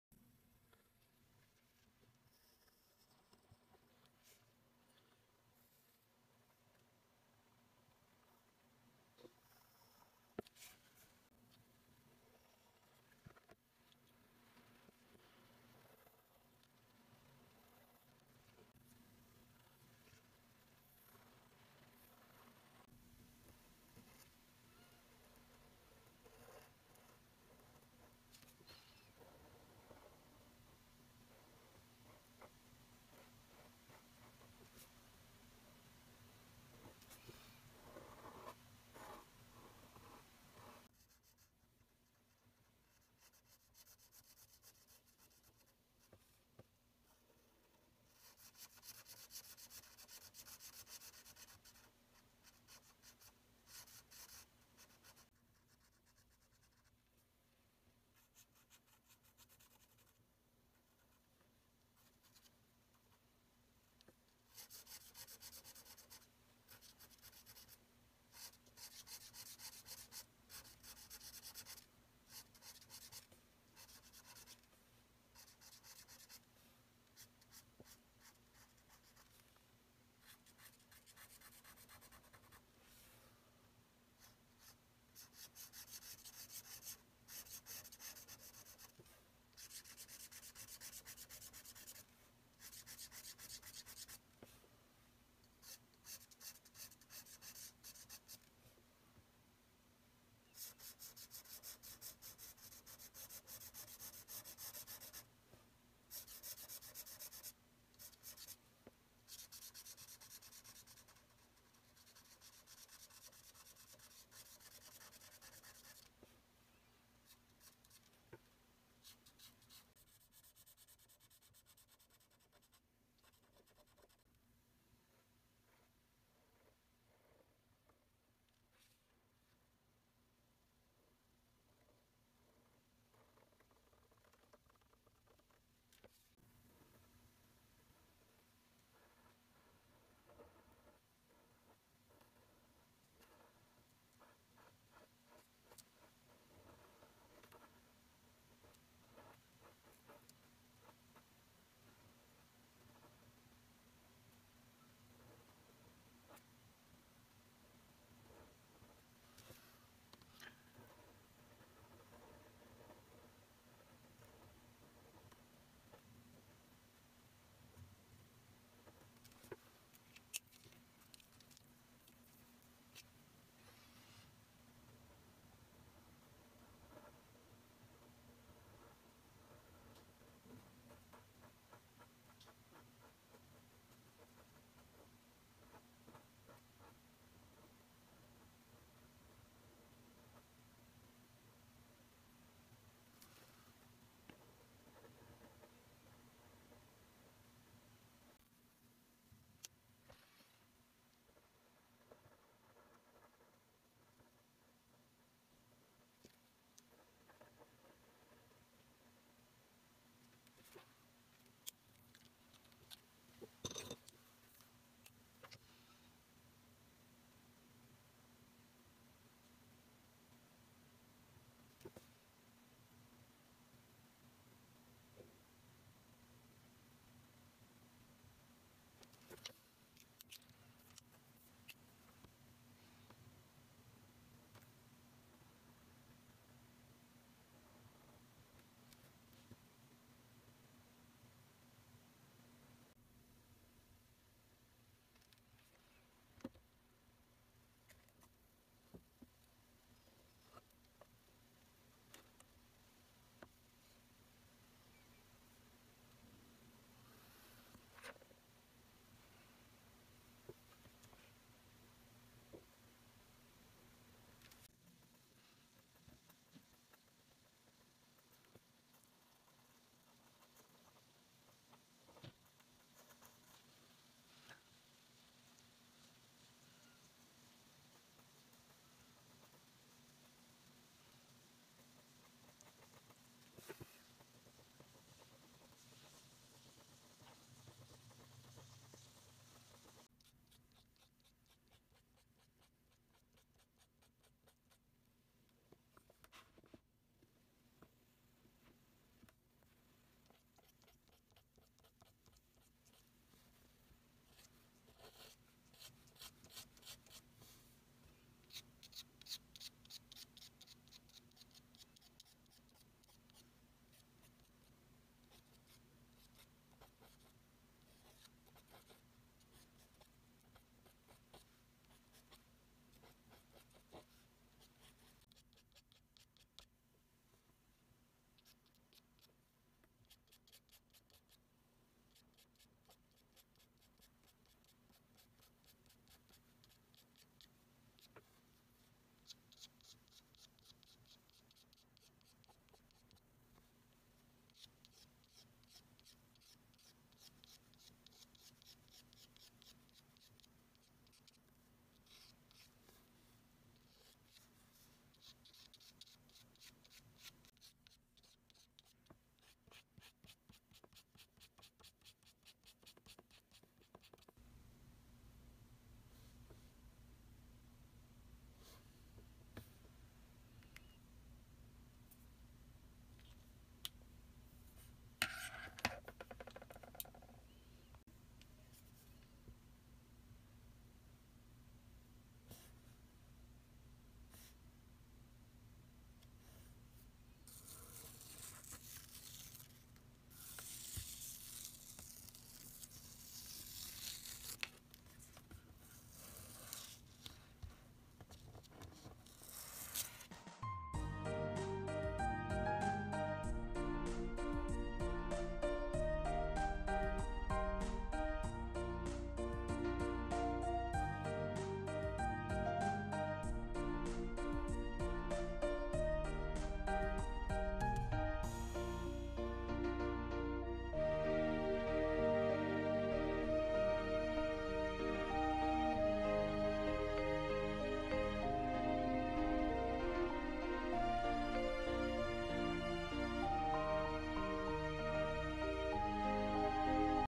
The sound of healing strokes《Beautiful fish》|art|drawing17|originalART THERAPY | Process
A simple and effective way to calm a bad mood。You can sleep well